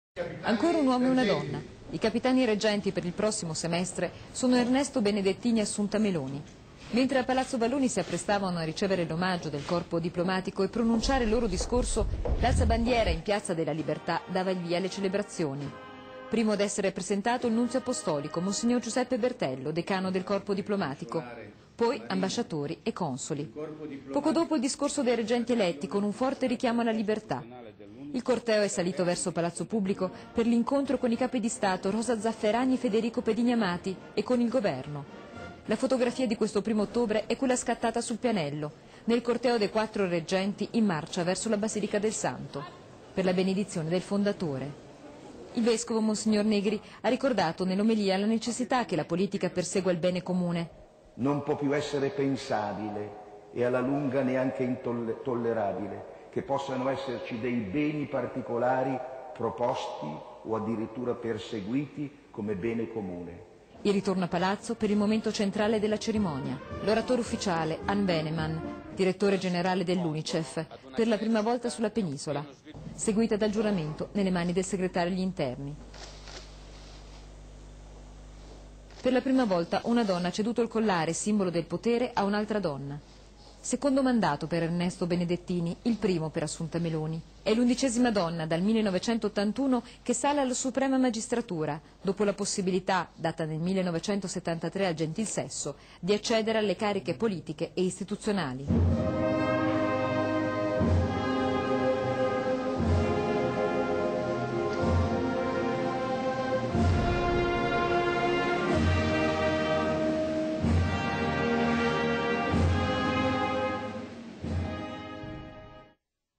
Insediati i nuovi Capitani Reggenti (RTV)
Da: San Marino RTV. Insediati i nuovi Capi di Stato. Sua Eccellenza Ernesto Benedettini e Sua Eccellenza Assunta Meloni salgono alla Suprema Magistratura. Nel messaggio di insediamento ribadiscono la priorità dellaccordo con lItalia in materia economica.